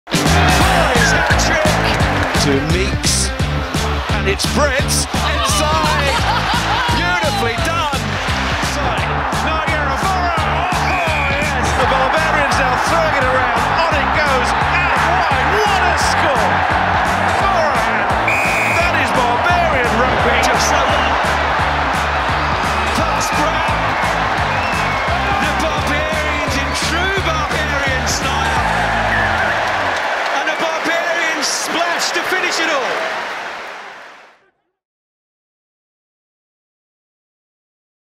The Killik Cup: Barbarians V Samoa
Tickets are on sale from 22/09/2021

The world-famous Barbarians are back and will take on Samoa this autumn at Twickenham for the Killik Cup on Saturday 27th November – 2.30pm Kick Off.  The Barbarians will be jam packed with stars from across the globe and look forward to welcoming fans back to live matches, for what always promises to be an electric game of International Rugby. So, anticipate some big hits, lots of skills on show, a thrilling game of rugby and a fun filled day out with friends and family guaranteed.

Australia Head Coach, Dave Rennie and Wales Assistant Coach, Jonathan Humphreys will take charge of the Barbarians and three experienced world class players have also been confirmed recently, the Argentine former Captain, Pablo Matera and Springboks Duane Vermeulen and Malcolm Marx.
A day of high-octane international rugby is expected at Twickenham in this historic double header event, with Barbarian Women taking on a Springboks Women’s XV following the men’s match at 5.15pm.